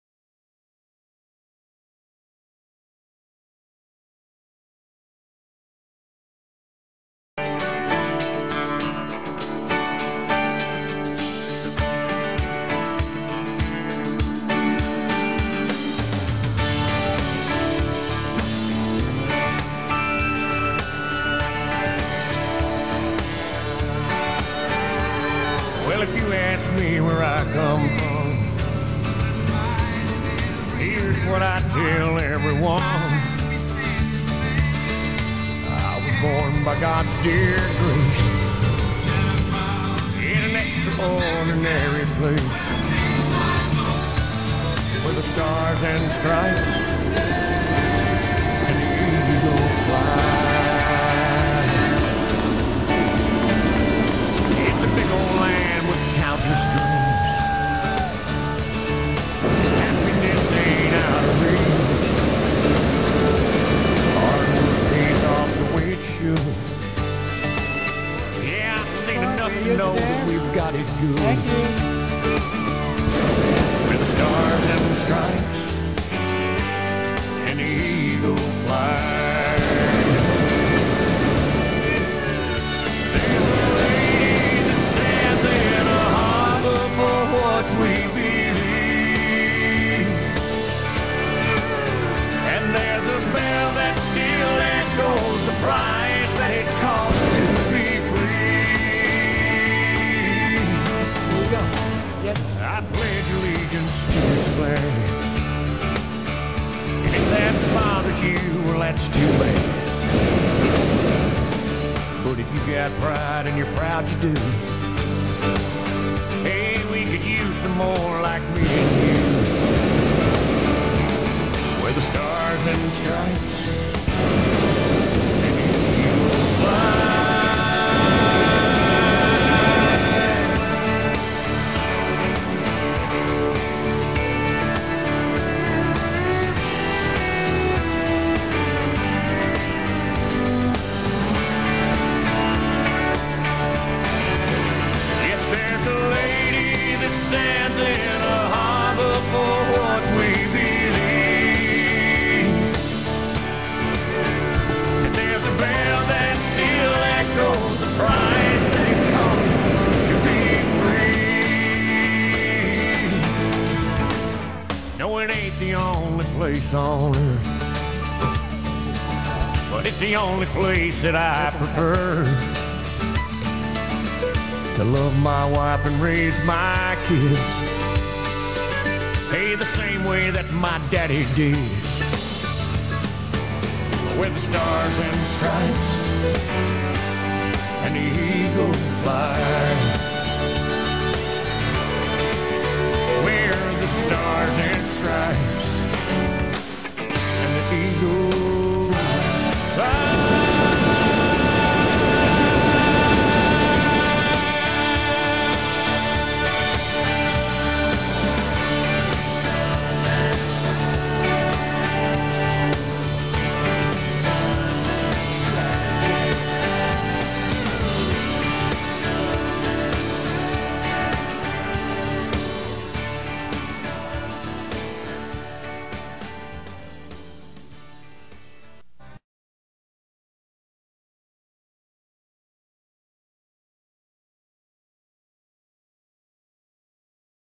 Sept 11th, 2017 US Flag Hot Air Balloon Remembrance Flight
As long as the weather is good, Barry flies the world's largest US Flag on 9/11.  This is the 2017 flight.